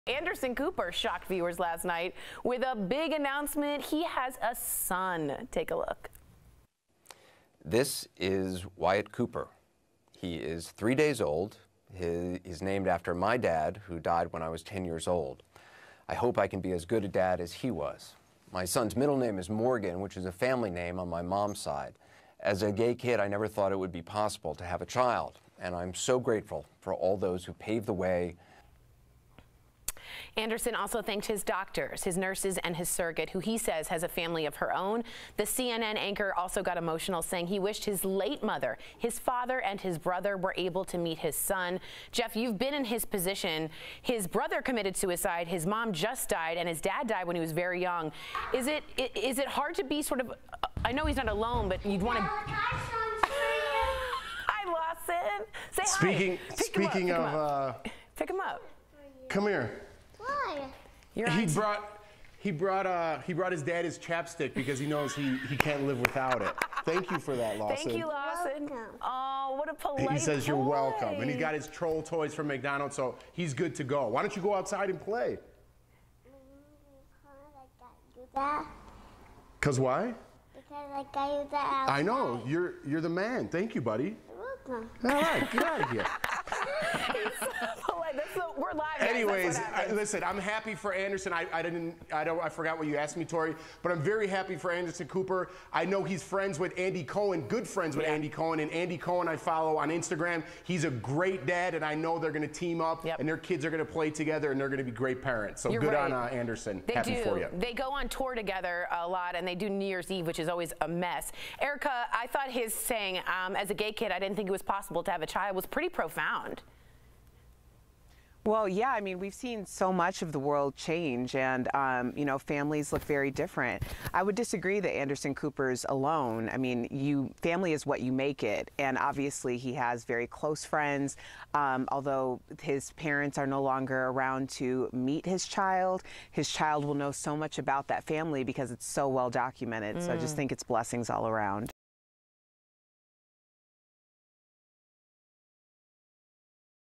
Anderson Cooper Welcomes Baby Boy Via Surrogate
CNN's Anderson Cooper is now a father. He named his son Wyatt Morgan Cooper.